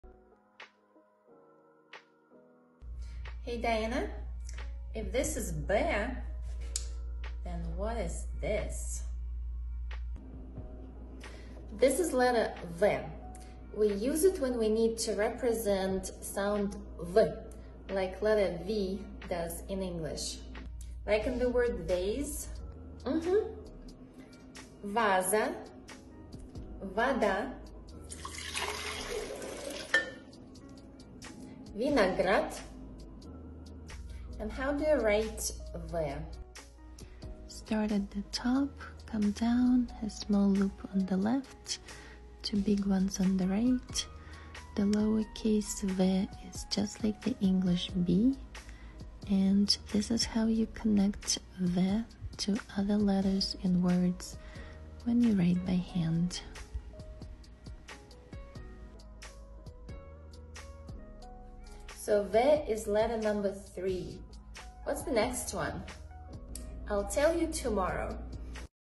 Russian Alphabet Marathon - How to pronounce letter "B" in Russian 🔤
Russian Alphabet Marathon - Third letter is "B"
It is pronounce as "Veh" , like in a word "Vase".